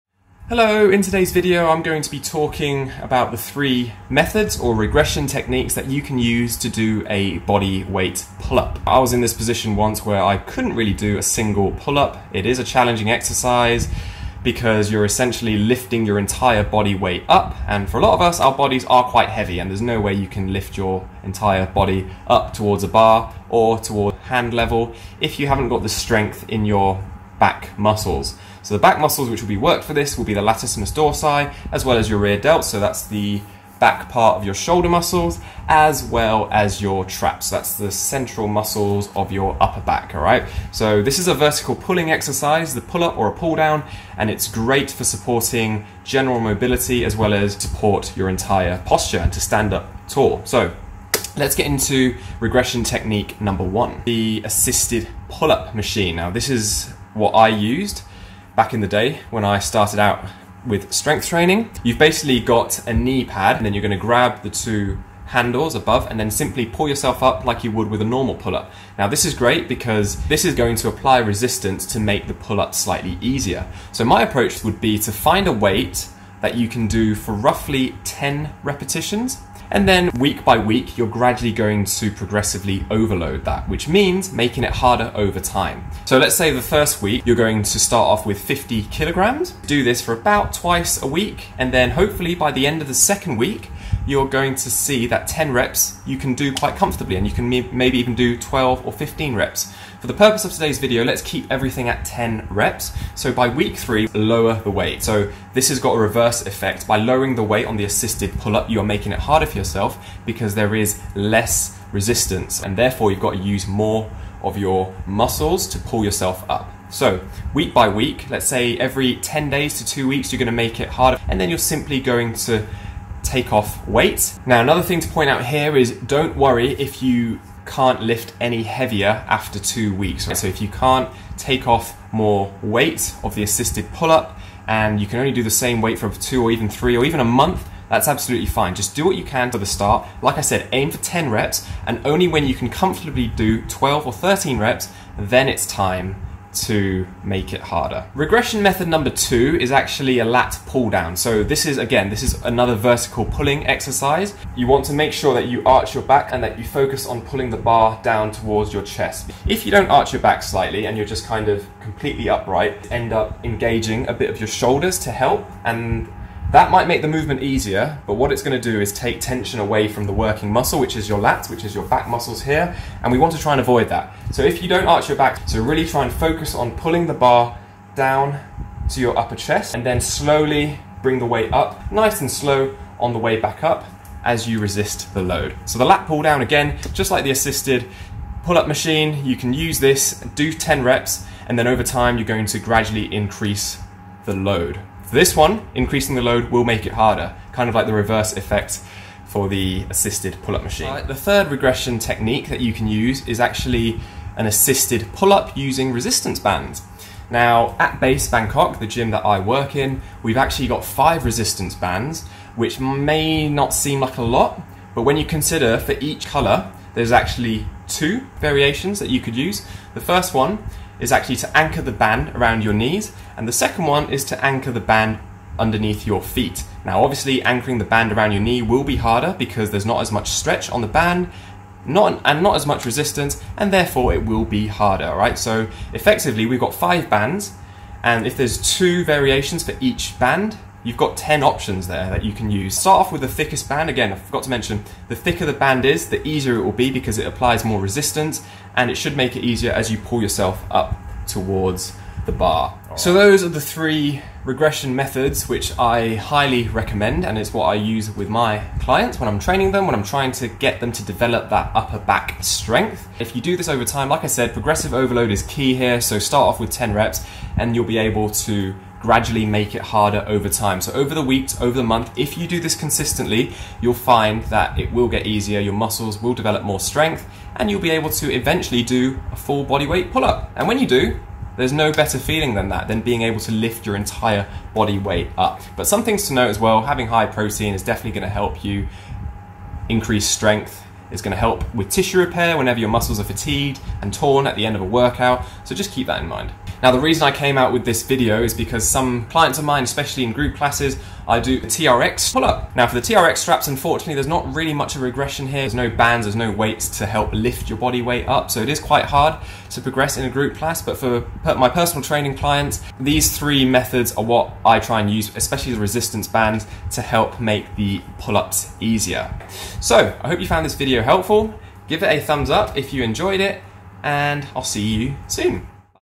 Pull up Regressions for beginners
*** Reupload *** (Originally Published 4 October 2022)
If you struggle to do Pull ups, then this video is for you! I go through the three methods you can use to make Pull ups slightly easier. With consistent training and progressive overload, the back muscles will get stronger to the point where you can get close enough to lifting your entire body up towards your hands!